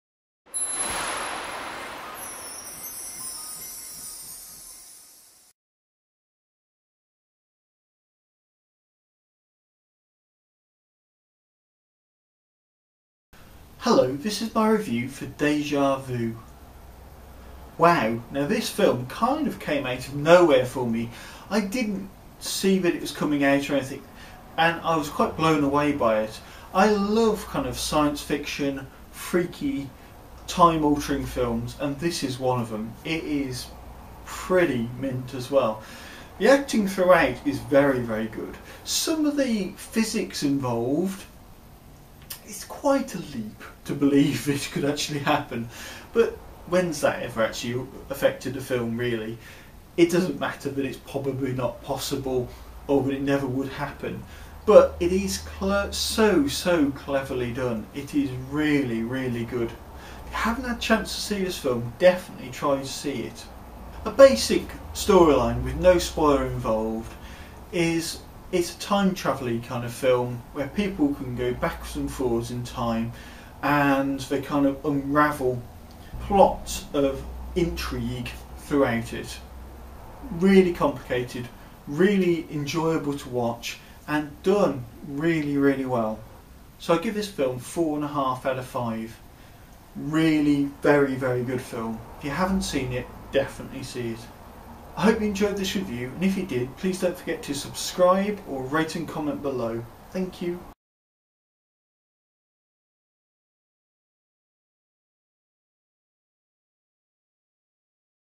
Deja Vu Review - Latest Film Reviews
A short video vlog review of the new film Deja Vu